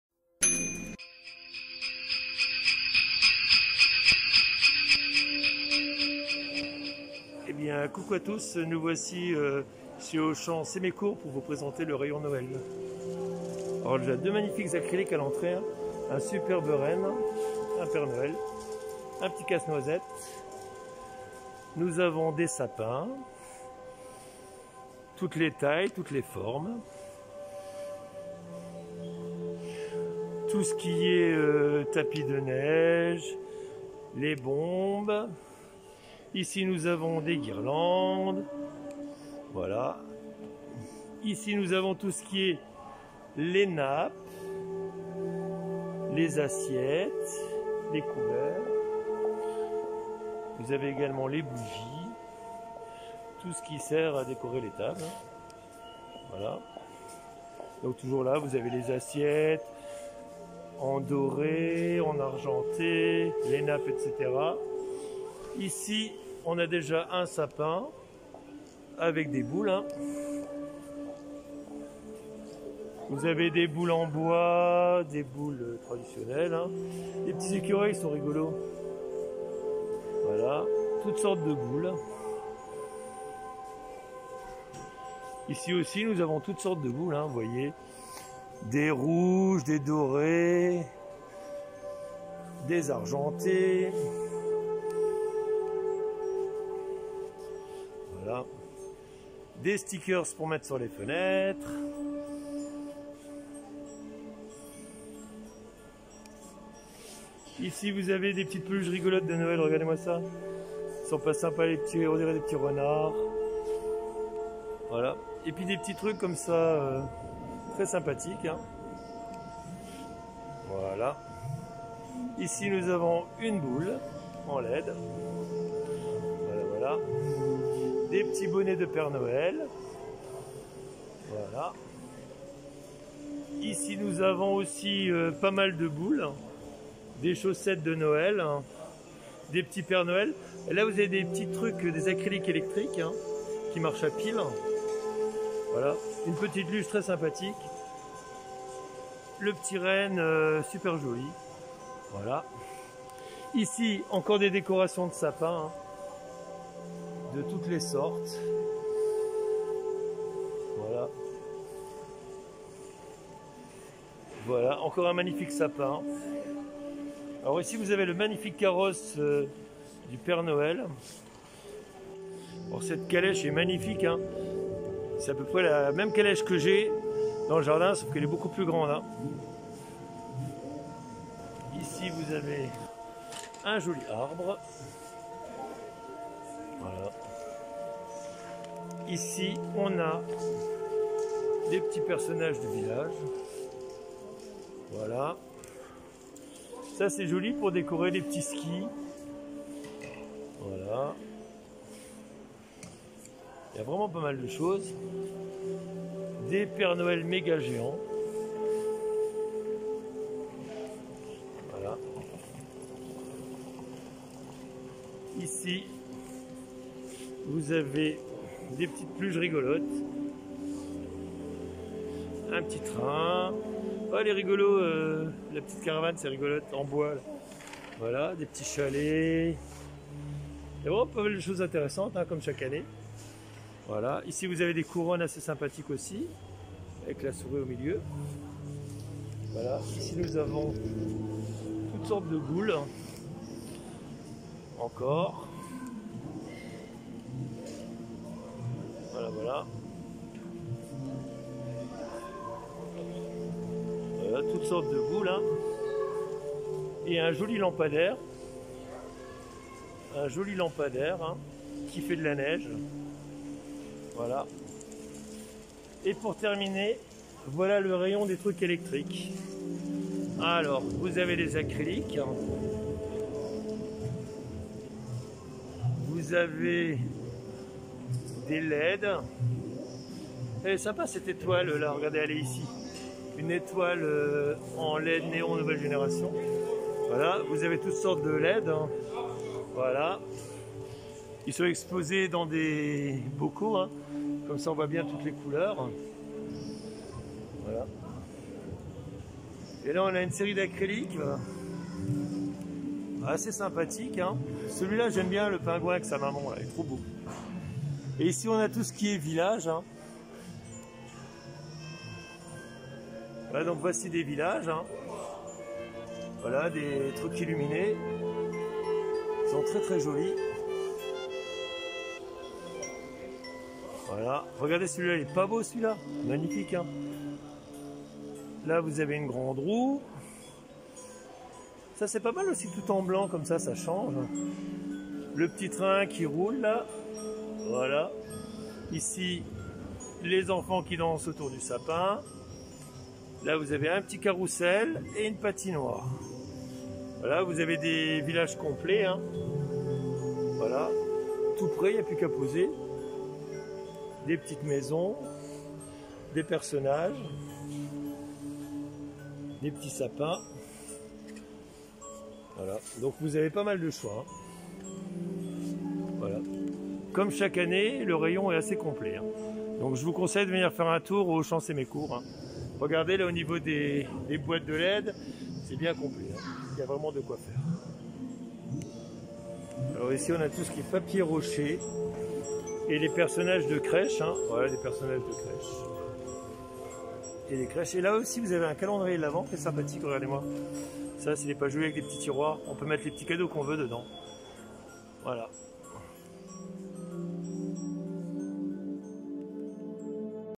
Noël 2021 chez Auchan Sémécourt
Le rayon est toujours assez complet avec pas mal de choix au niveau des décorations électriques ; j'ai d’ailleurs acheté le joli acrylique de la maman pingouin avec son bébé.